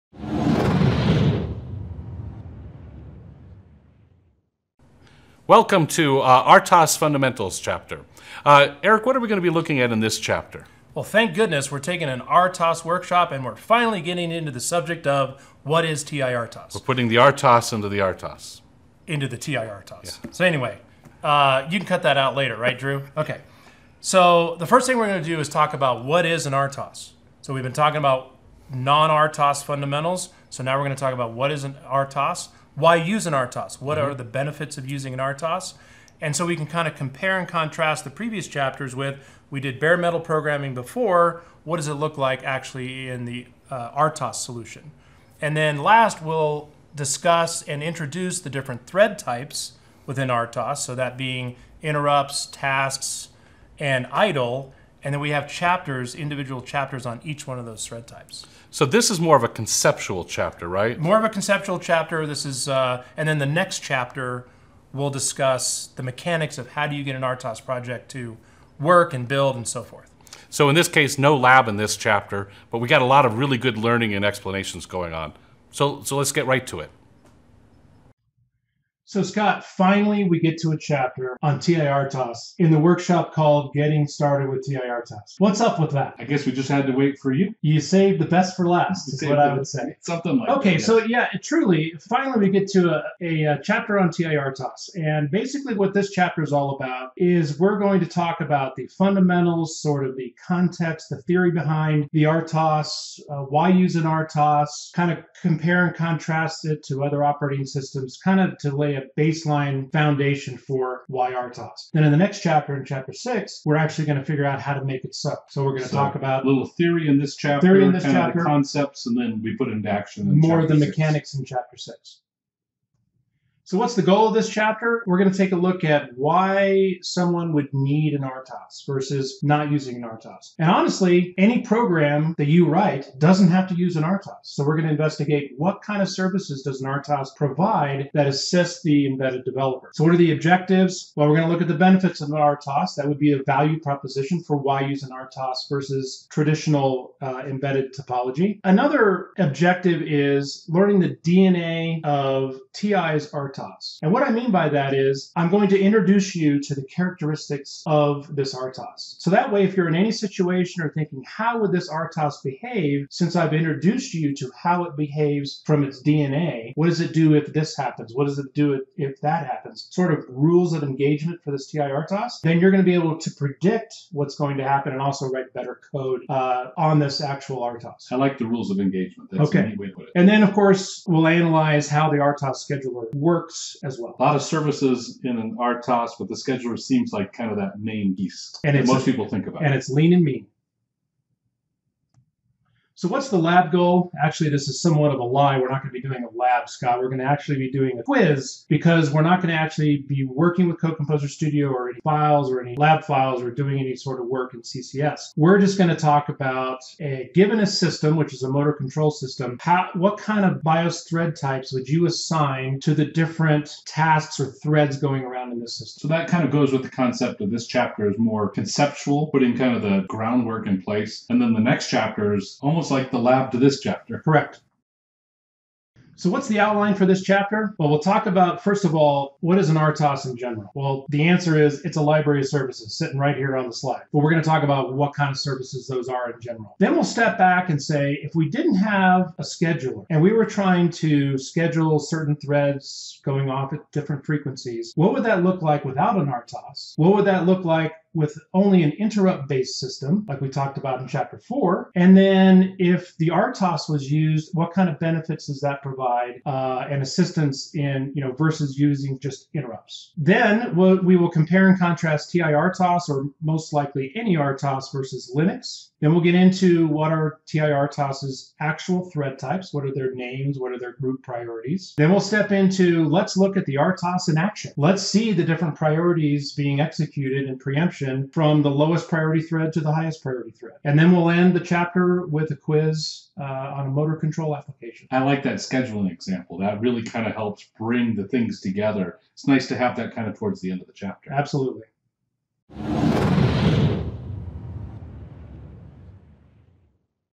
Getting Started with TI-RTOS: Chapter 5—TI-RTOS intro
Learn more about the "Getting Started with TI-RTOS" workshop.

In this video, Eric Wilbur and Scott Specker of *Embedded Advantage [1]*
describe the objectives, agenda, and lab goals for Chapter 5 of
the *Getting Started with TI-RTOS [2] *workshop.

Chapter 5 covers these topics and answers these questions:

*RTOS – Library of Services*

 * Why use an RTOS?
 * Explain RTOS vs bare-metal programming
 * What is the difference between RTOS and Linux?
 * Compare/contrast Linux and RTOS
 * What are the TI-RTOS thread types?
 * What is the list of TI-RTOS Services?

*Why use an RTOS Scheduler?*

 * What is an RTOS Scheduler?
 * How does an RTOS Scheduler work?
 * What are the benefits of using an RTOS?

*TI-RTOS DNA*

 * What is the DNA of TI-RTOS and how does it behave in general?

*Linux vs RTOS – Compare/Contrast*

*TI-RTOS Thread Types *

*System Timeline & Scheduling Example*

 * What is the timeline of events when using an RTOS?
 * Can I see the TI-RTOS scheduler in action?

*Quiz: TI-RTOS Threads*

 * How do I assign thread types to my application?